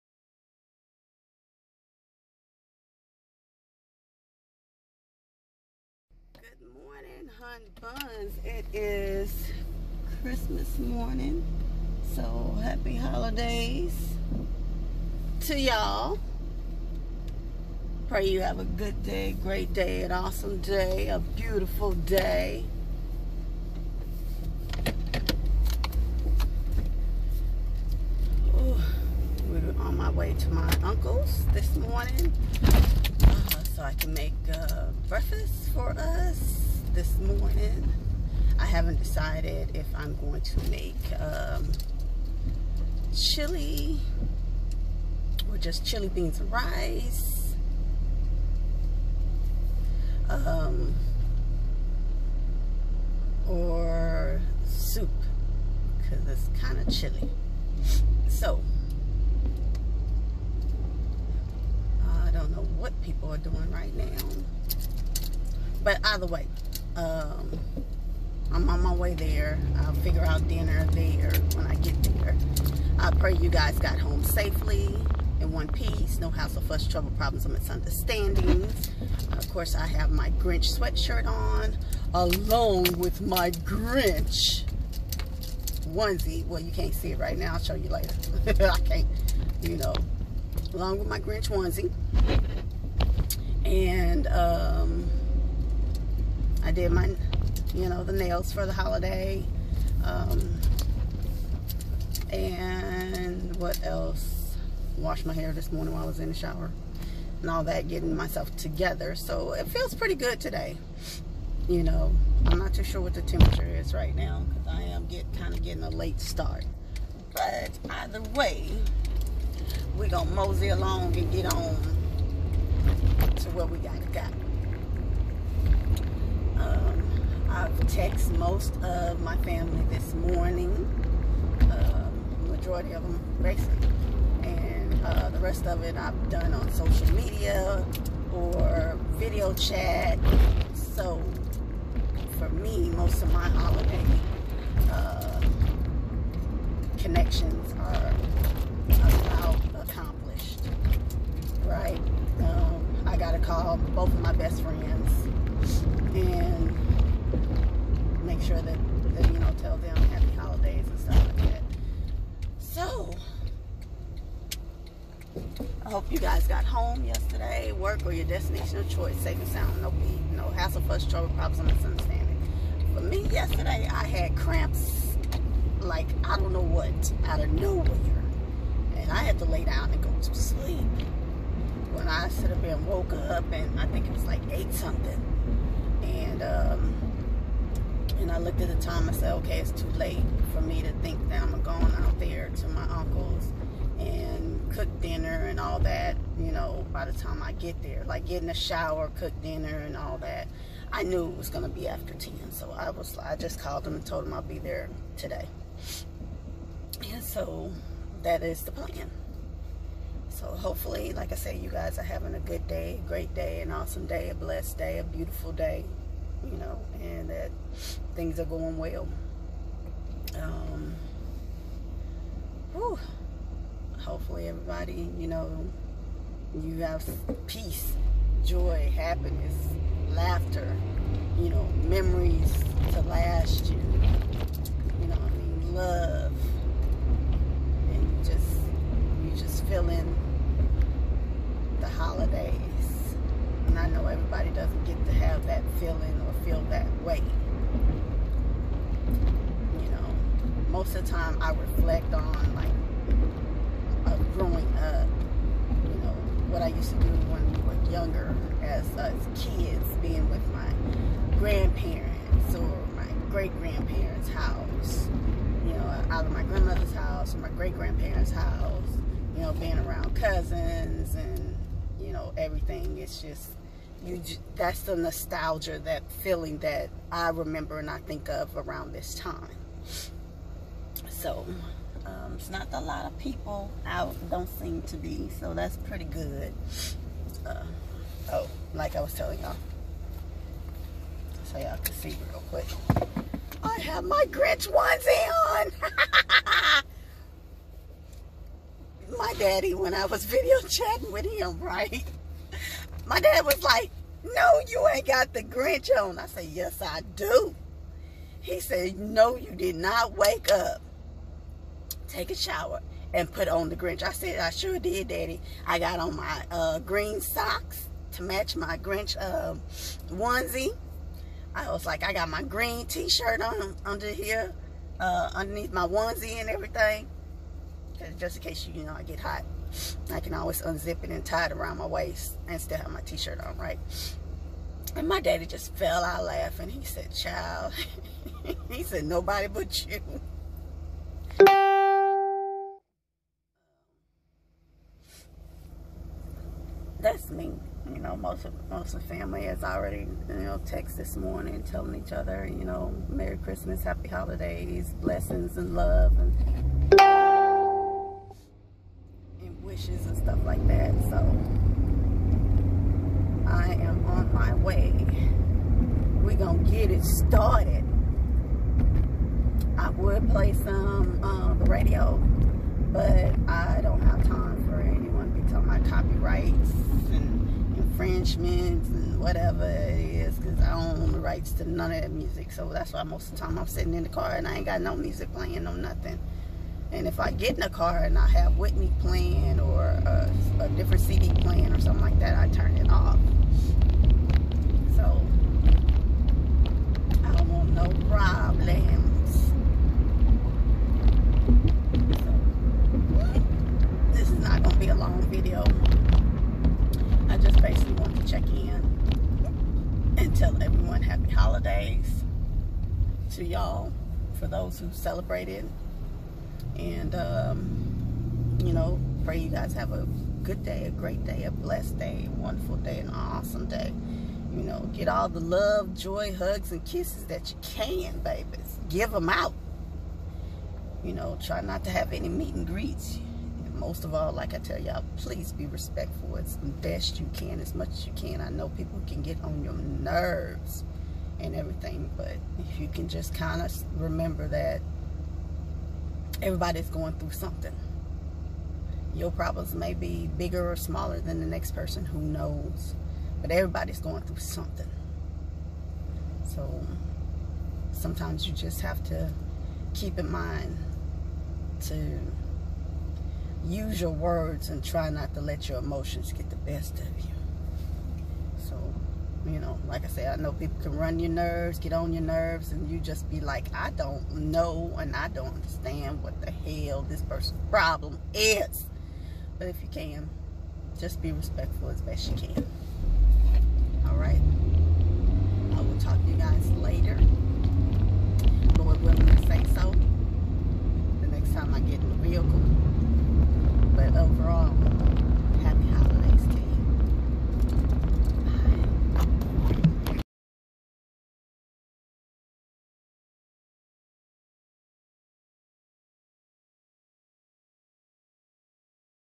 Let's ride hun buns!!! Episode 228 Happy Holiday's
Happy Holiday's hun buns!!! Have a safe, blessed,  loving,  fun, full if laughter and memories day.

Come on get in the car and buckle your seatbelts while we talk on my way to work, going home going to events or even road trips. Beep Beep let's go hun buns!!!